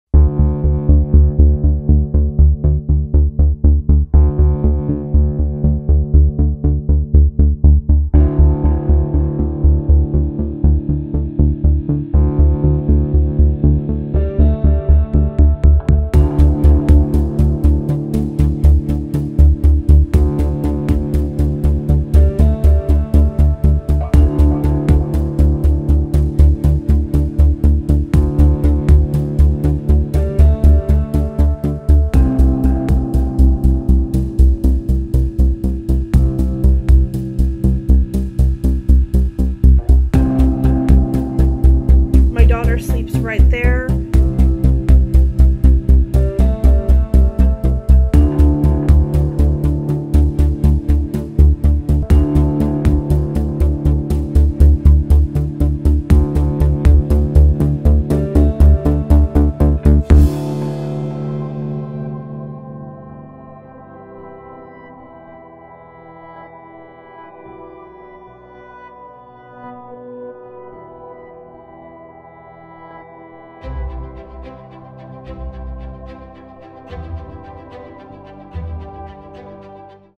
Bullets strike just feet from a baby's crib in a military family's home
A Navy family living off base in Virginia Beach narrowly escaped injury when their house was struck by bullets from a drive-by shooting that left with 17 bullet holes in the wall just feet from where their 8-month-old daughter lay sleeping.